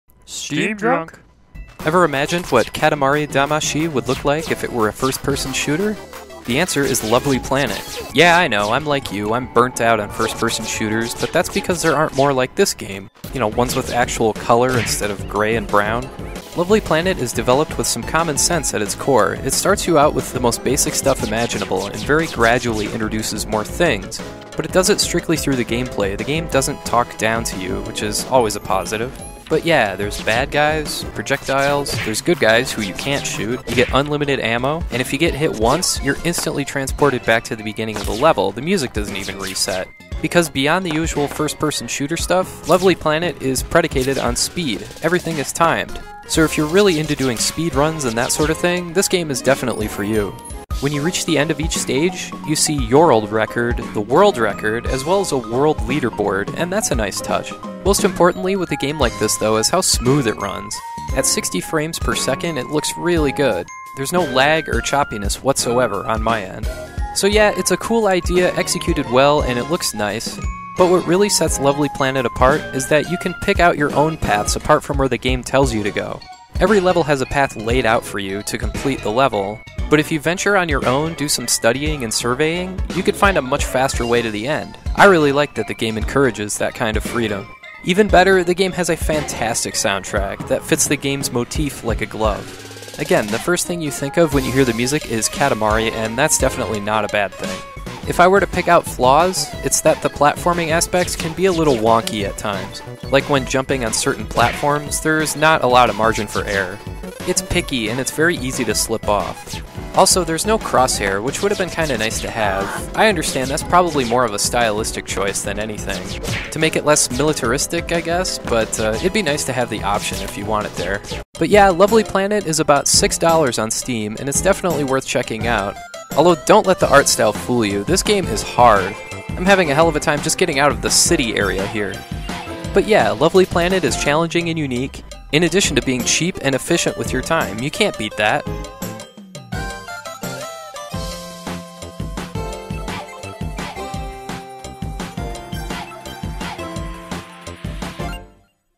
Lovely Planet review - Steamdrunk
A first-person shooter that's something out of the Katamari Damacy universe...